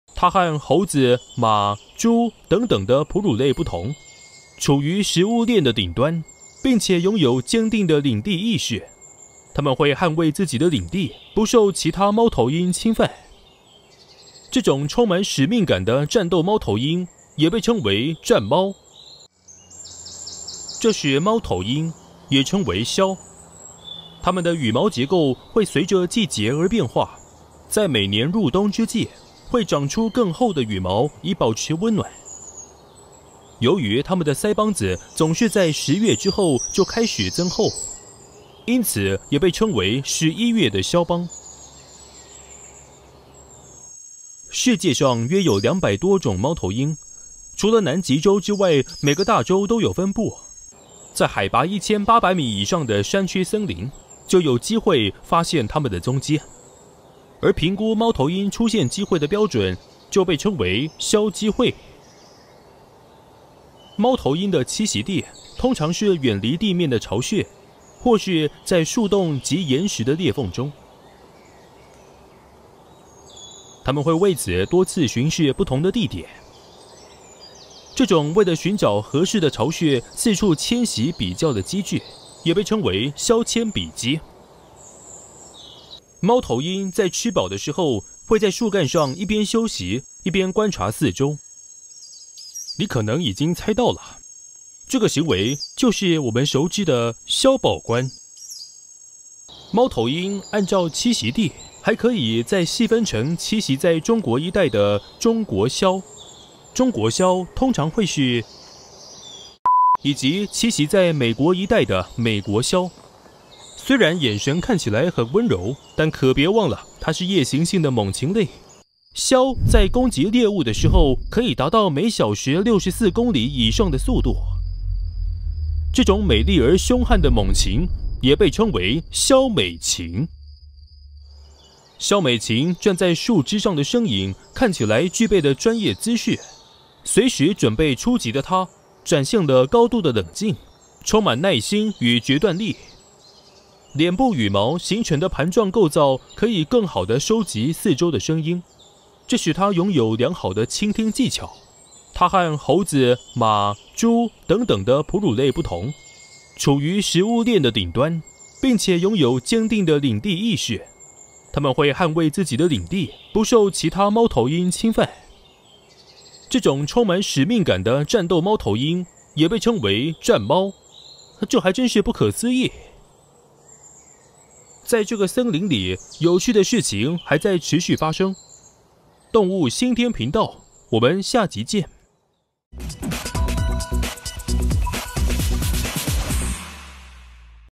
偷臭猴馬豬！「鴞美禽」動物介紹片秀高級黑 網笑岔氣釣到本尊：不可思議－全民話燒
娛樂中心／黃笙報導

2024立委及總統大選進入白熱化階段，就連各陣營的副手也備受外界矚目，鋒頭甚至與搭檔有過之而無不及。近日知名配音員「星期天」，更新戲仿動物頻道的系列作品，標題名為〈深入探索：貓頭鷹〉，以嚴肅認真的口吻介紹物種特性，卻一本正經參雜不少逗趣的諧音哏，其中不可思議的「鴞美禽」更是釣到本尊現身留言。